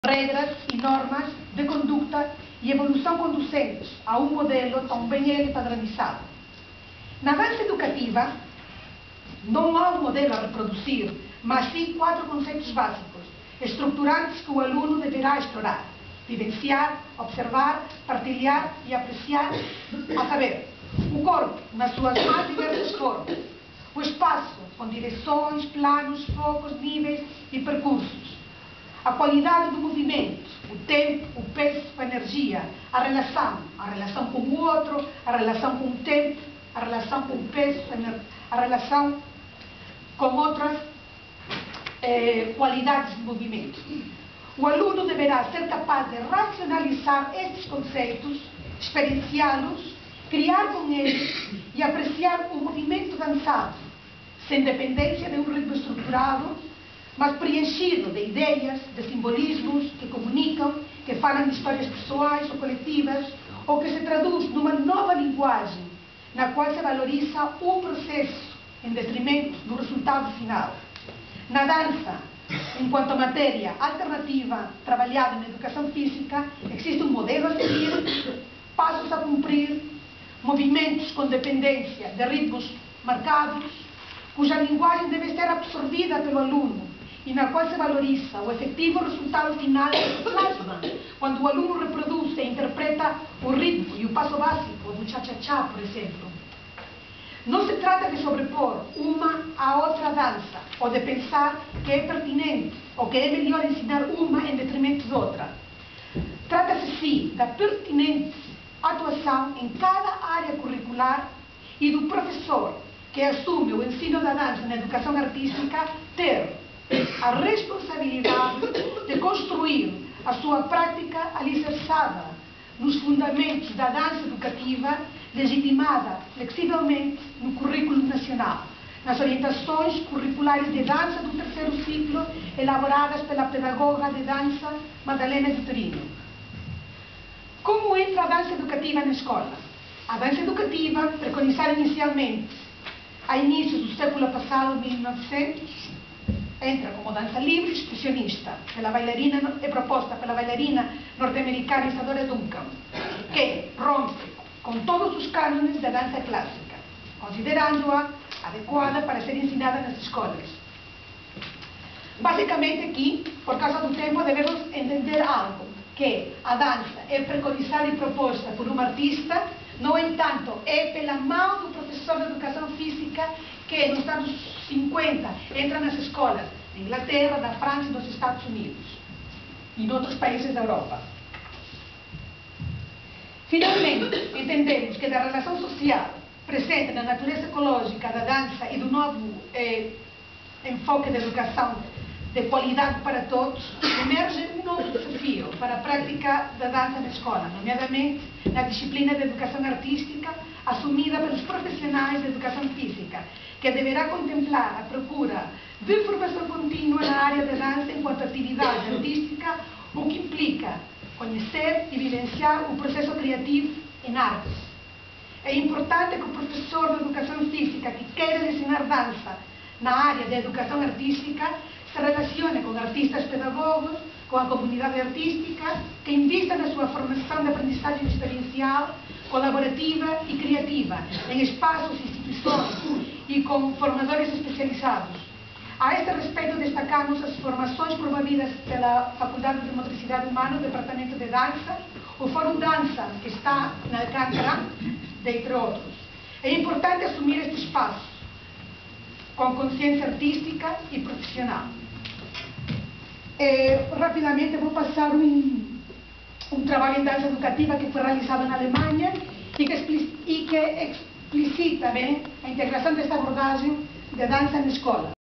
9º Congresso Nacional de EF -- Luisa Alegre (parte 1)
9º Congresso Nacional de Educação Física "Desafiar o presente, renovar o futuro", de 1 a 3 de Março de 2013, na FMH.
Novo desafio para a prática de dança na escola: Reflexão pedagógica
Luisa Alegre